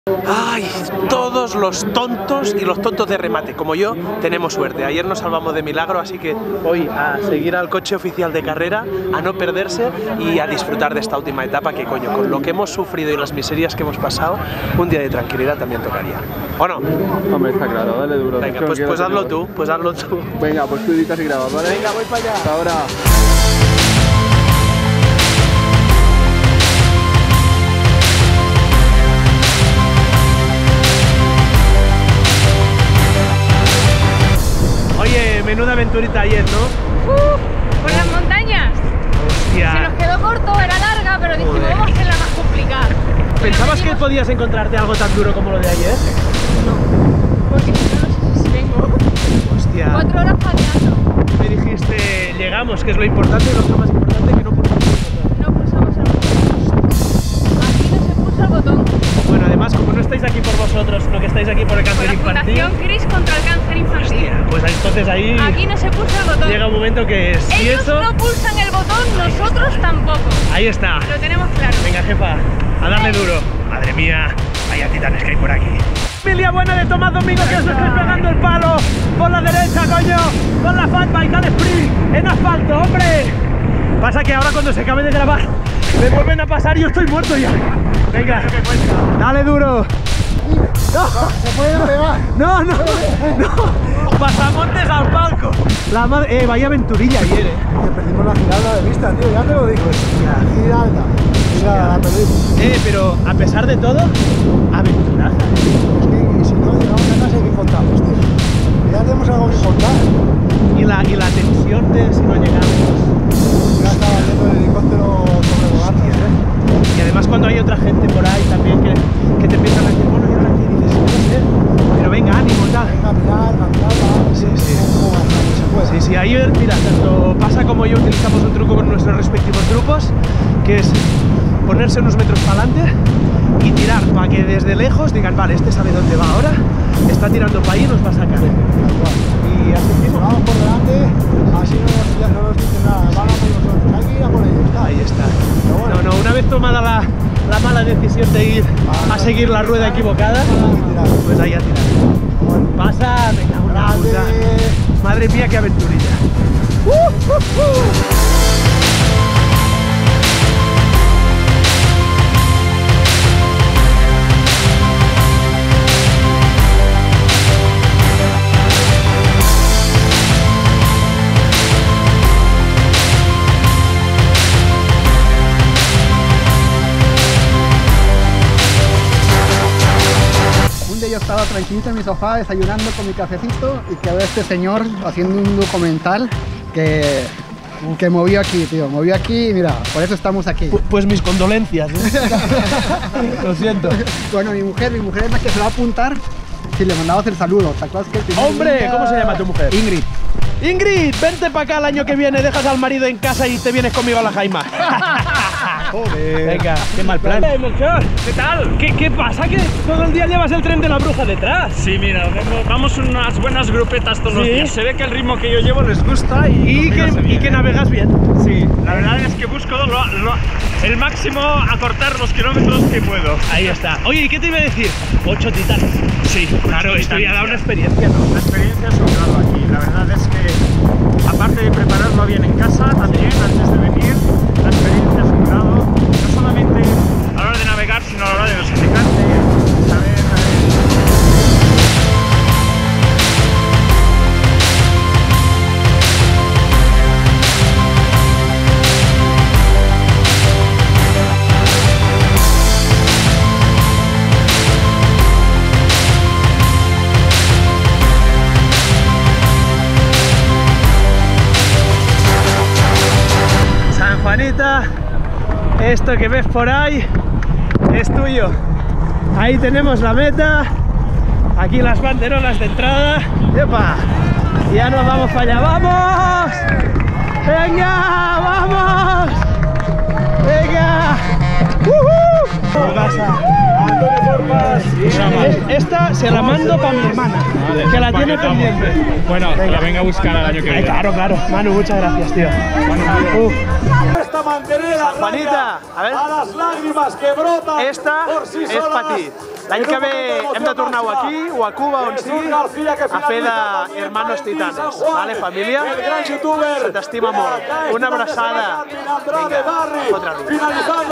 Titan Desert 2017 Etapa 6 A La Saca!
Titan Desert 2017, etapa 6 conseguida! Hermanita, VA POR TI! Sé que has pedaleado a mi lado :) Titan Desert A LA SACA!!! El desierto es una especie de espejo que te devuelve tu propia imagen: si vienes tocado puede ser un infierno, si llegas con ganas de disfrutar, se convierte en el paraíso! Y este año viví las dos caras. Contento además con esa 6ª plaza en categoría fatbiken!